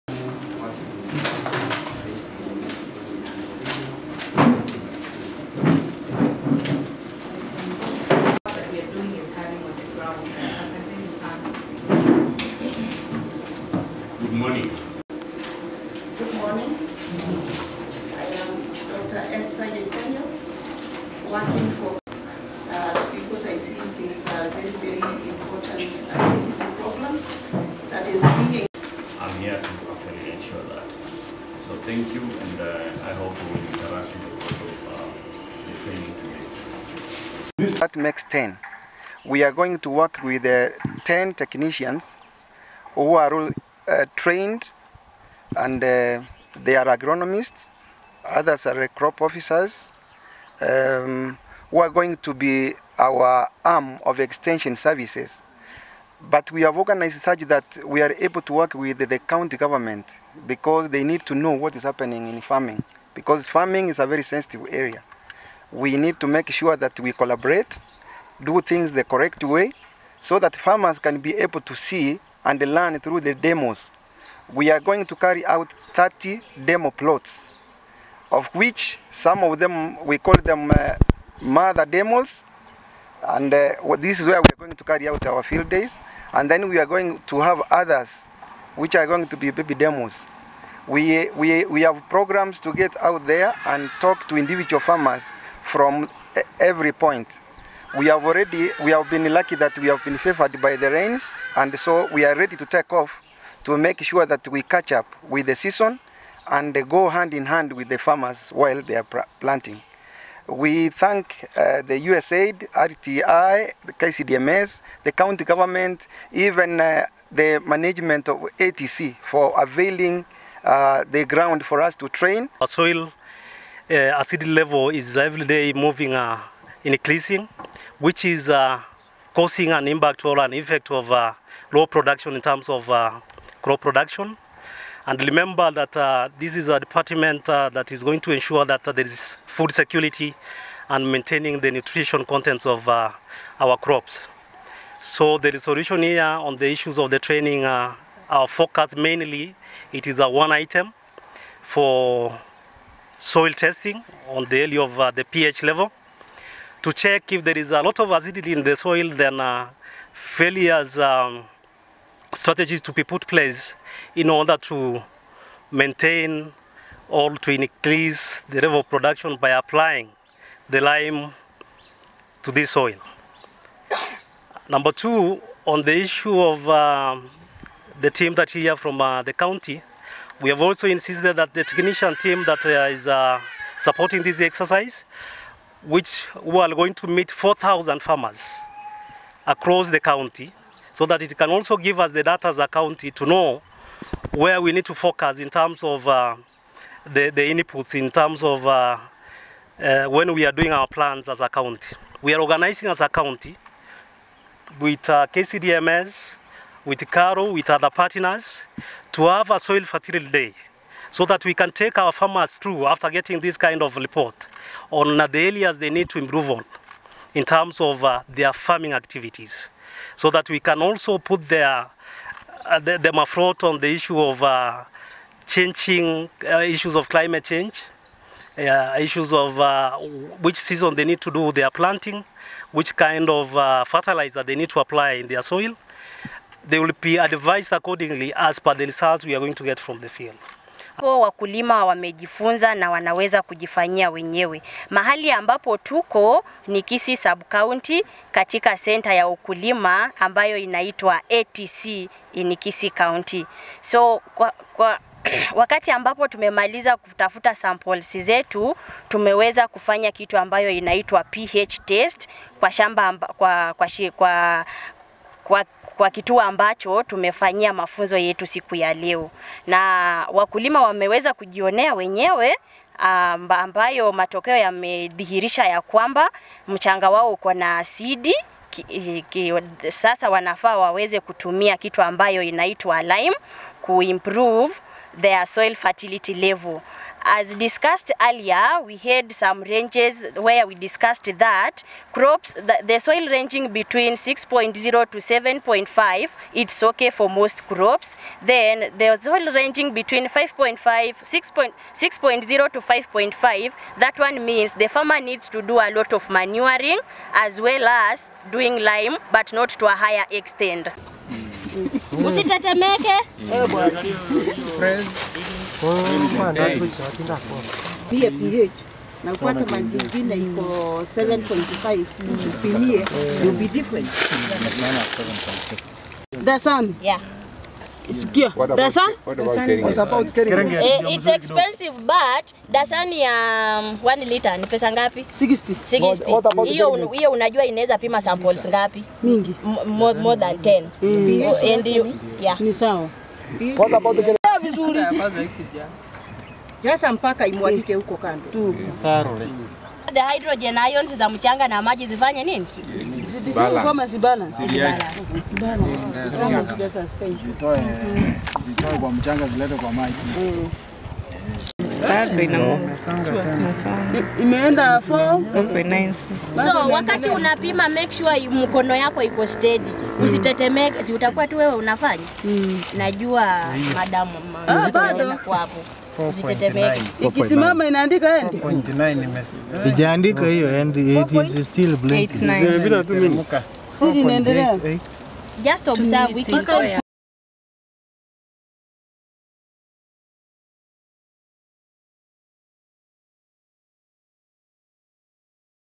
Kisii subsidy programme commences
A stakeholder-driven programme on enhanced agricultural production,  poverty reduction and malnutrition has commenced in the county.